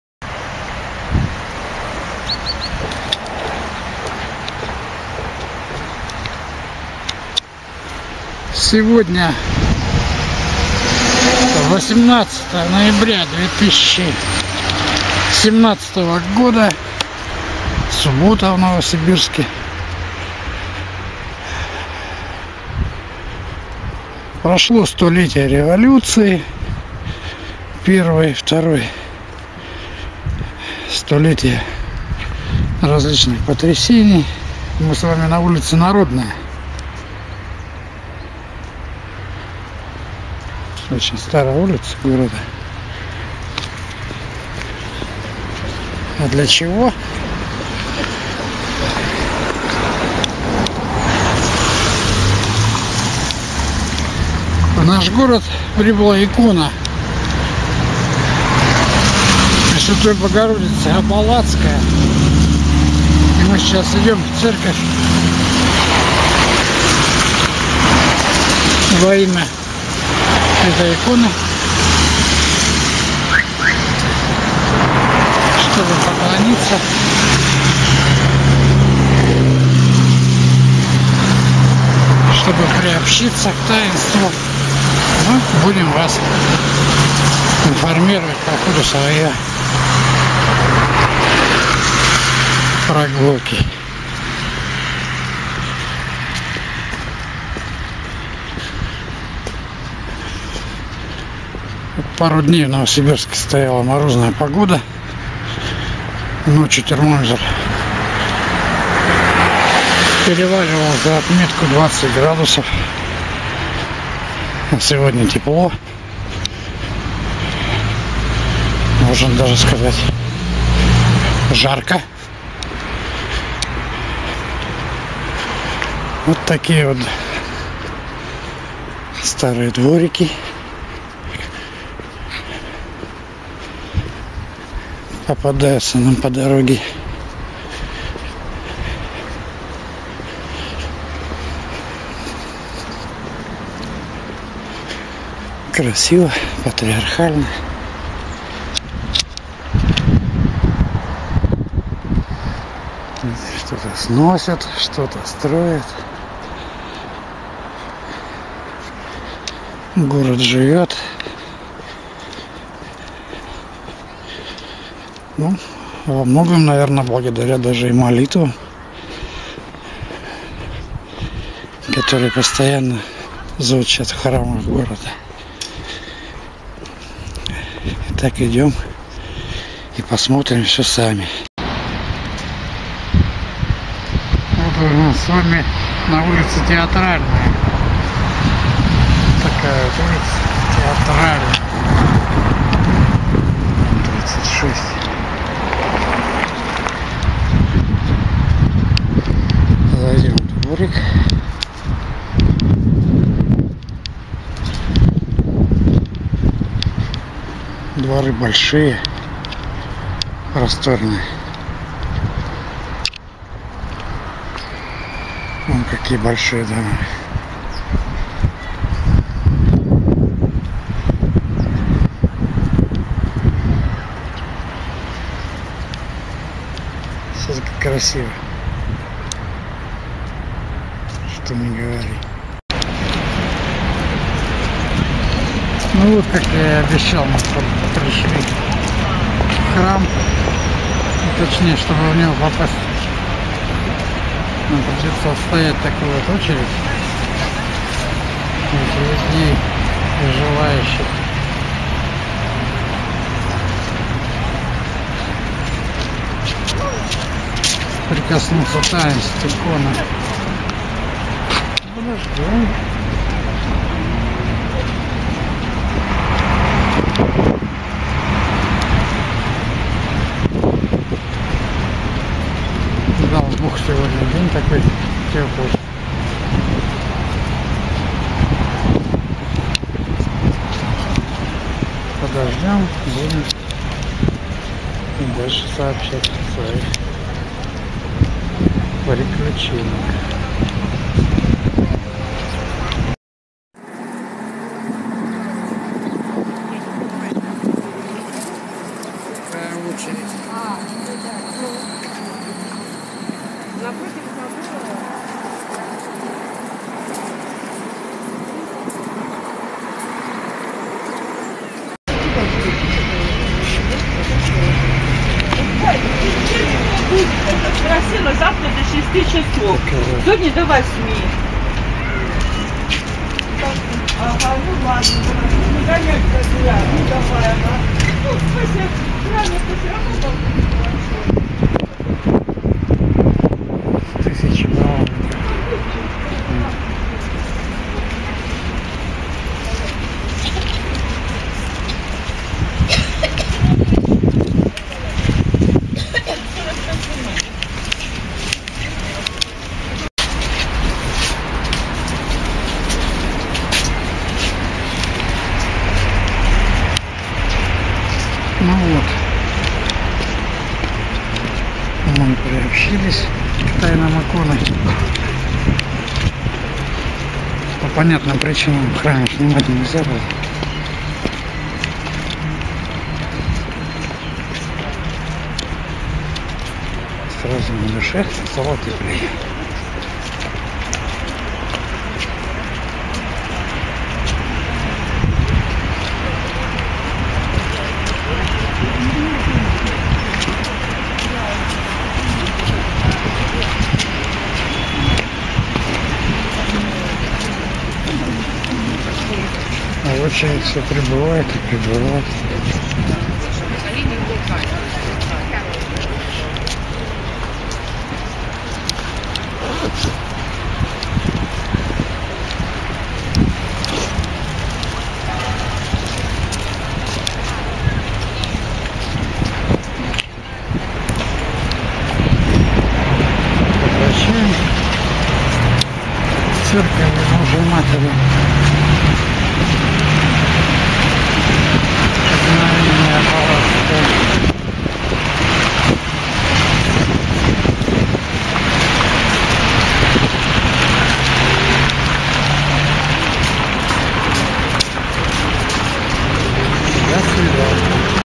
В храм во имя иконы божьей матери Знамение Абалацкая.
18/11/2017 года, суббота. В храме выставлена икона Пресвятой Богородицы знамения Аболацкая и мы идём поклониться святыне.